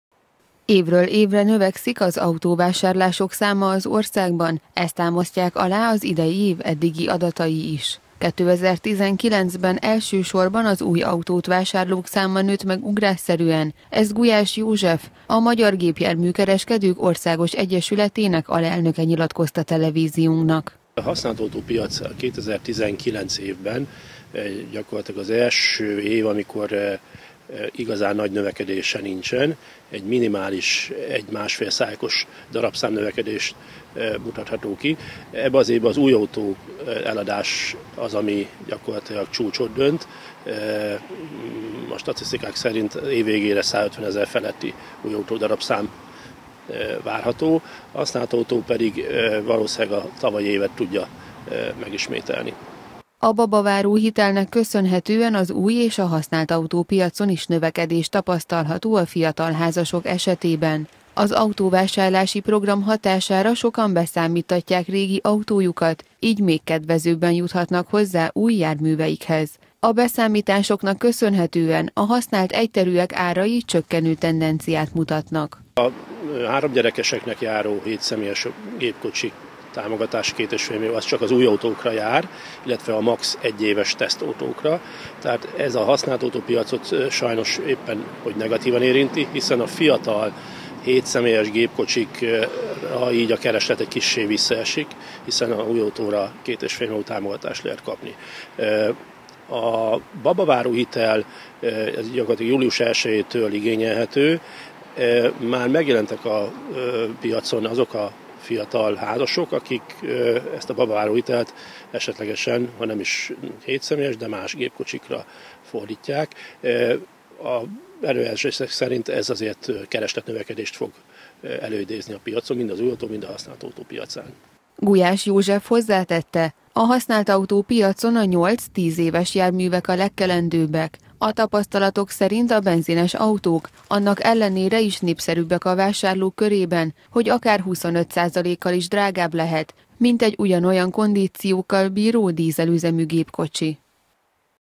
Növekedés az autópiacon - Kölcsey Tv
Ugrásszerűen megnőtt az új autók iránti kereslet 2019-ben.
A használtautó-piacon is növekedés tapasztalható, a babaváró-támogatást ugyanis sok fiatal házaspár fordítja autóvásárlásra. Bár egy használt benzines jármű több pénzbe kerül, mint egy dízel, mégis népszerűbb a vásárlók körében.